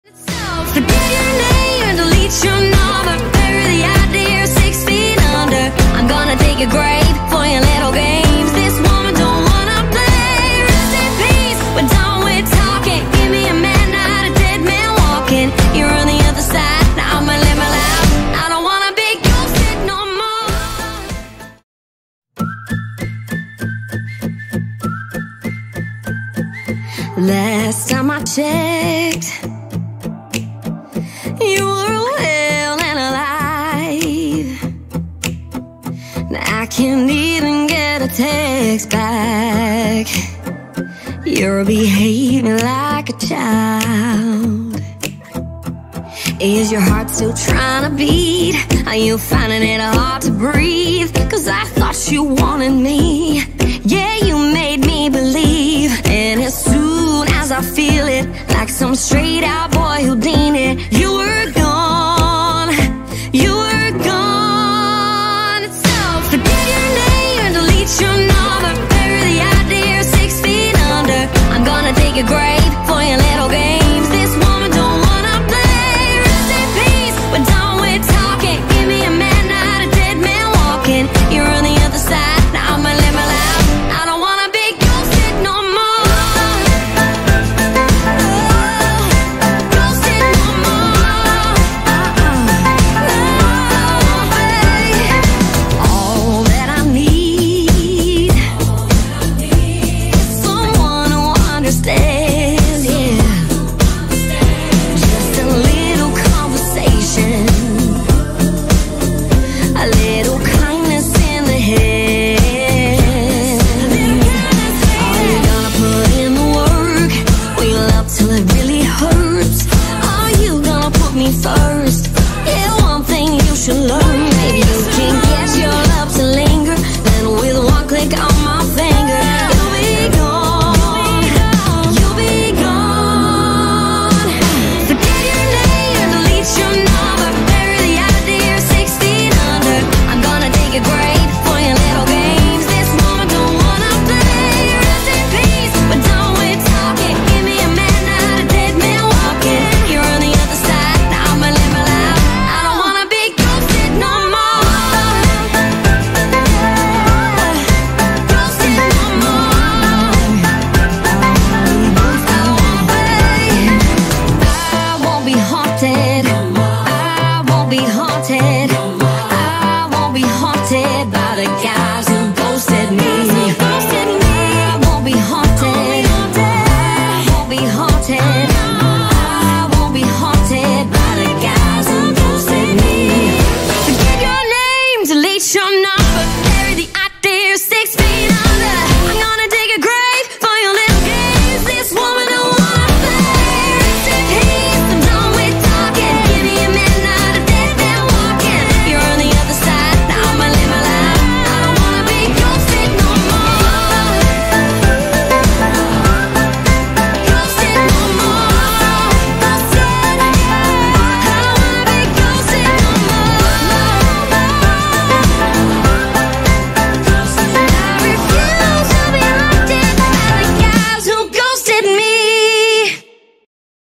Ghosted - Line Dance Demo
Line Dance Demo

Dance: Ghosted
Choreographer: Trent Duncan
Level: Improver
Count: 32
Walls: 4
Tags/Restarts: 2 restarts